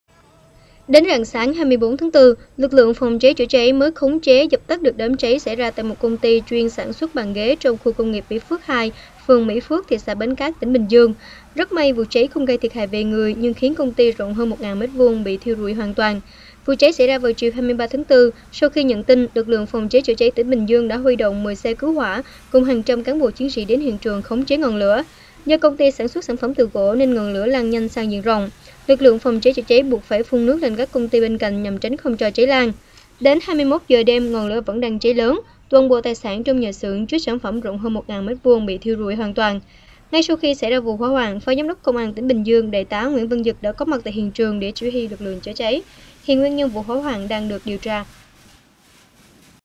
Hơn 1.000 m2 nhà xưởng bị thiêu rụi trong vụ cháy lớn | NLĐTV
Đến rạng sáng 24-4 lực lượng PCCC mới khống chế, dập tắt được đám cháy xảy ra tại Công ty TNHH White Feathers International, chuyên sản xuất sản bàn ghế trong khu công nghiệp Mỹ Phước 2, phường Mỹ Phước, thị xã Bến Cát, tỉnh Bình Dương. Rất may, vụ cháy không gây thiệt hại về người nhưng khiến công ty rộng hơn 1.000m2 bị thiêu rụi hoàn toàn